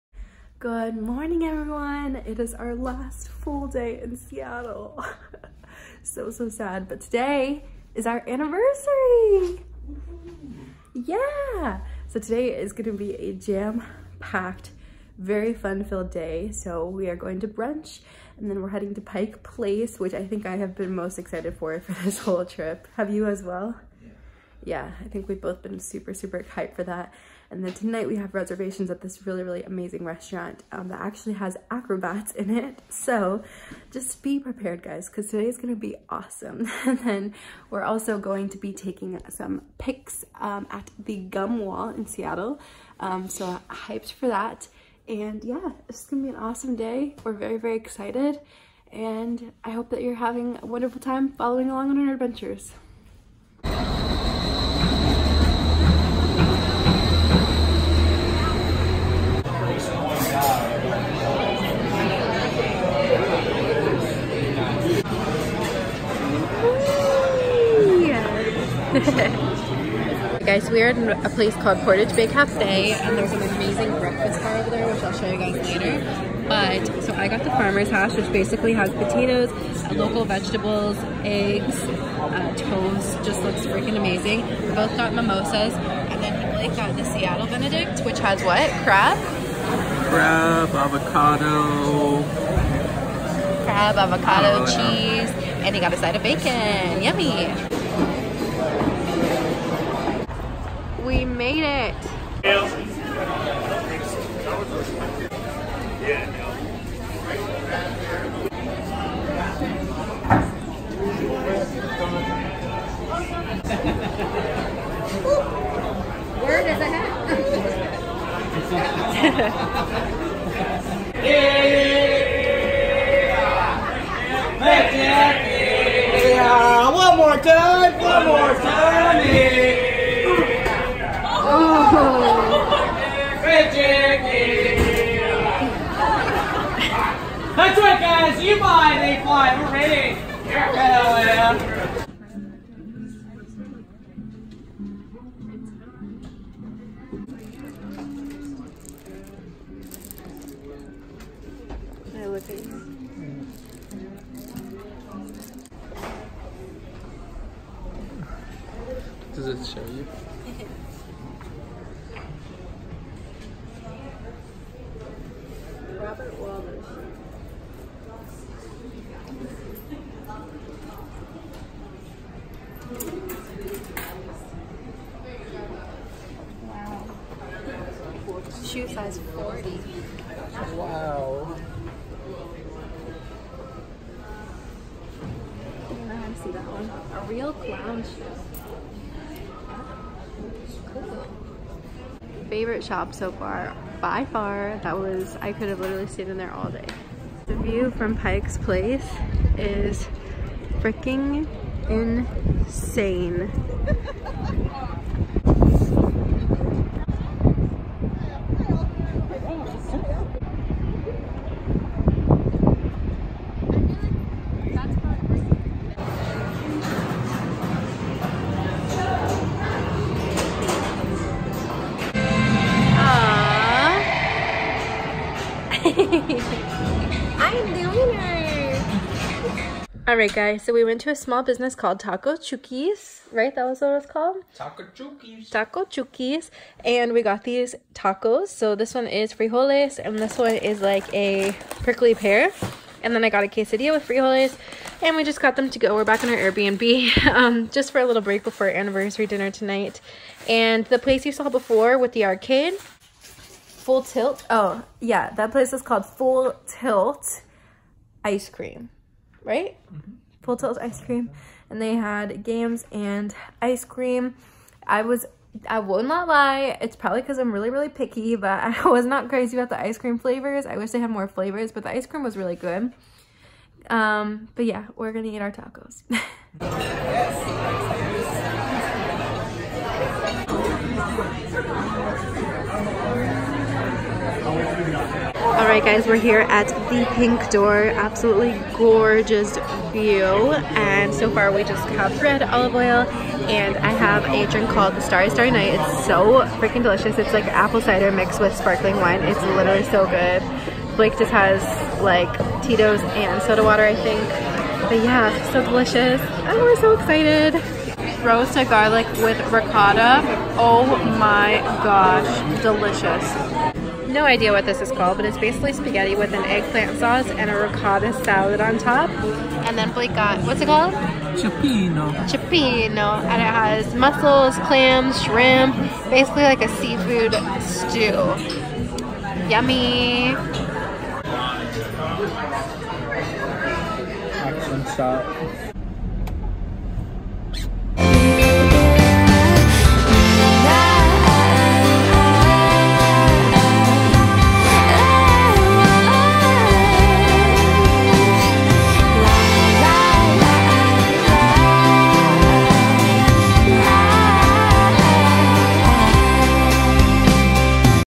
Pike Place Market, Shoe Museum, + Our Anniversary! | Seattle Travel Vlog
Hey everyone!
Here is the third of our Seattle Vlogs! This week we went to Pike Place Market, the Famous Fish Market (You buy 'em, we fly 'em!), the shoe museum, to our anniversary dinner and anniversary brunch!
What have you been up to?!

Marissa

SEATTLE LINKS:

(While I would  not personally recommend frontier, they are one of the cheapest options for budget travel)

Striped Headband

For your first month free of Care Of Vitamins, use code MPAYTN